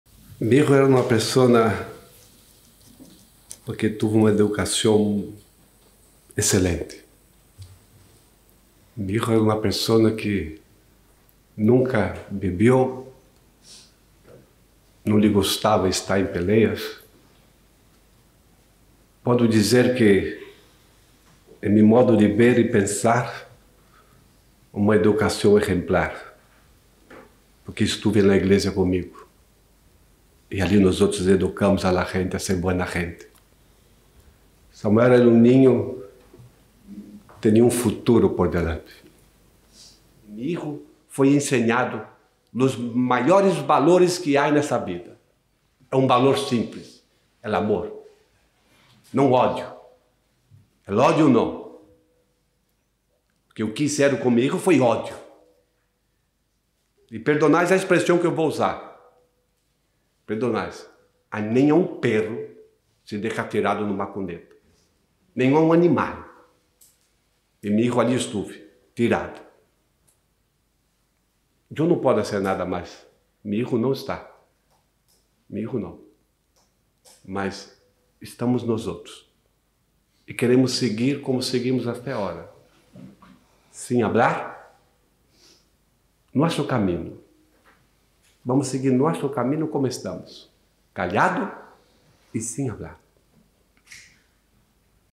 El padre de Samuel Luiz:"Lo que hicieron con mi hijo fue odio, ni a un perro se le deja tirado"
El padre de Samuel Luiz, Maxsoud Luiz, ha ratificado que su hijo nunca tuvo un problema cuando salía por la noche. "Nunca bebió, no le gustaba estar en peleas", ha expuesto, muy emocionado y con lágrimas, en una nueva sesión del juicio que se celebra en la Audiencia Provincial de A Coruña con Tribunal del Jurado.

(Fuente: Xustiza de Galicia)